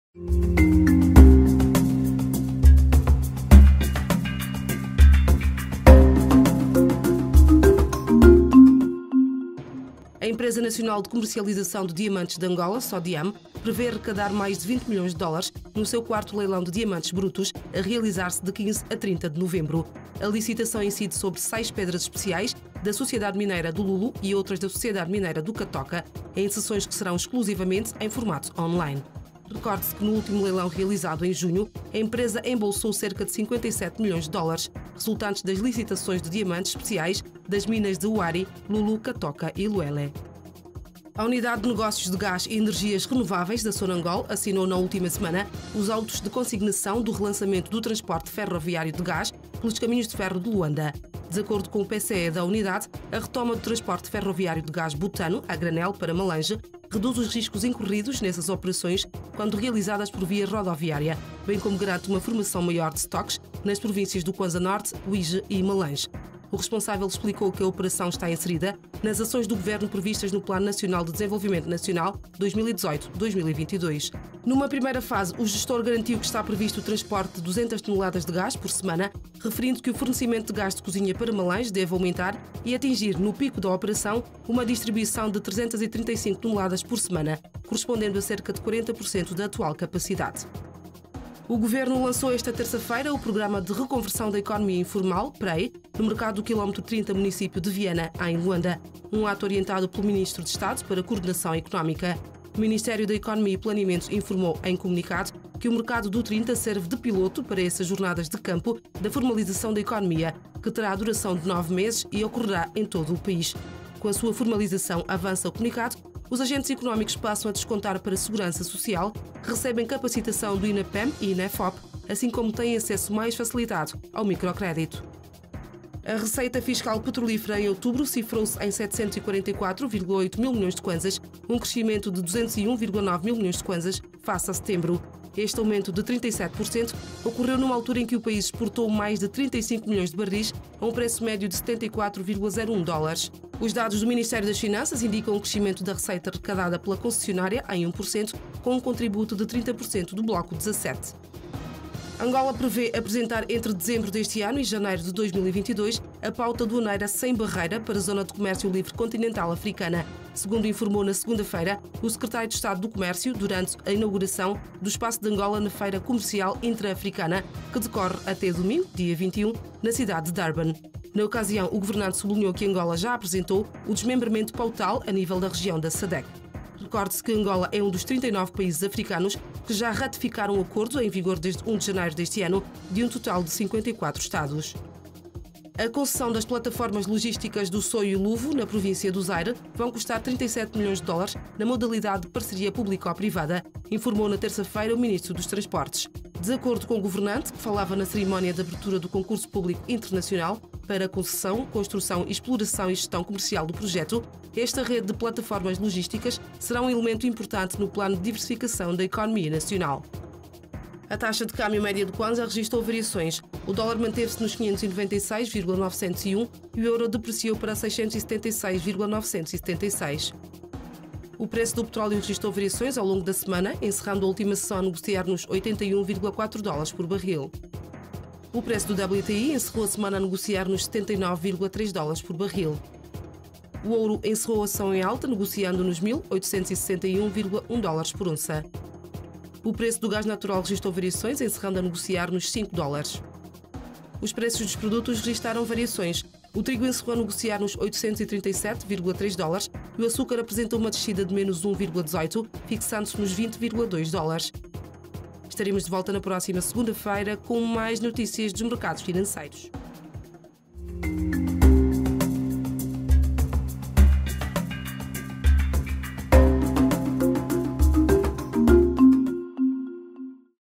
Mercados ao Minuto BAI: 15 a 20 de Novembro de 2021 (BAI)
Acompanhe mais um Mercados ao Minuto, com o resumo das principais notícias do mercado financeiro, de 15 a 20 de Novembro de 2021.